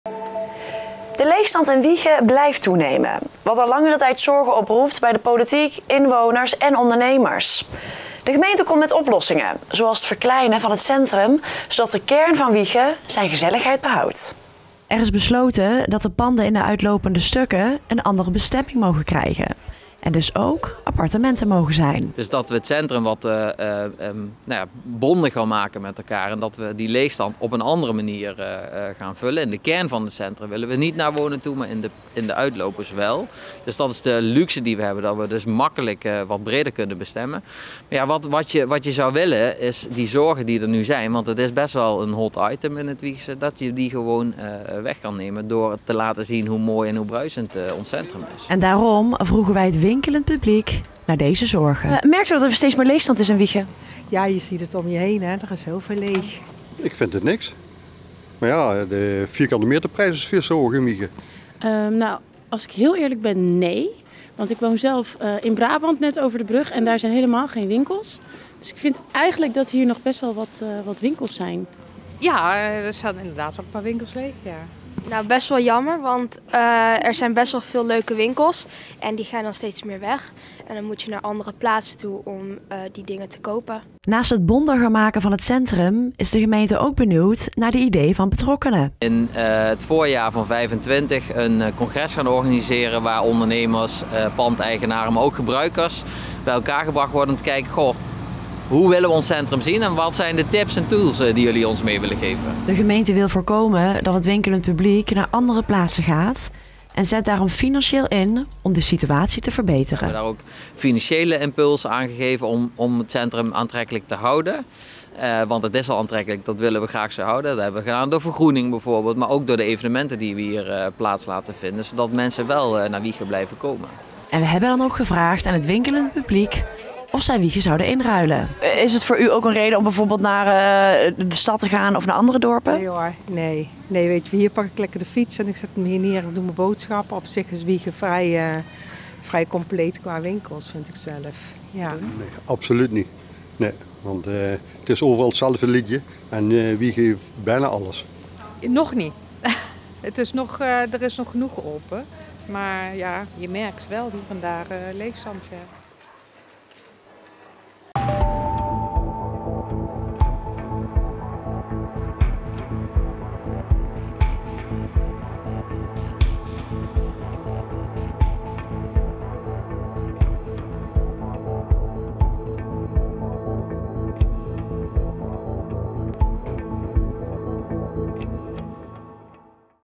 Wijchen in actie tegen toenemende leegstand || RN7 REGIONIEUWS
De leegstand in Wijchen blijft toenemen, wat al langere tijd zorgen oproept bij de politiek, inwoners en ondernemers. De gemeente komt met oplossingen, zoals het verkleinen van het centrum, zodat de kern van Wijchen zijn gezelligheid behoudt.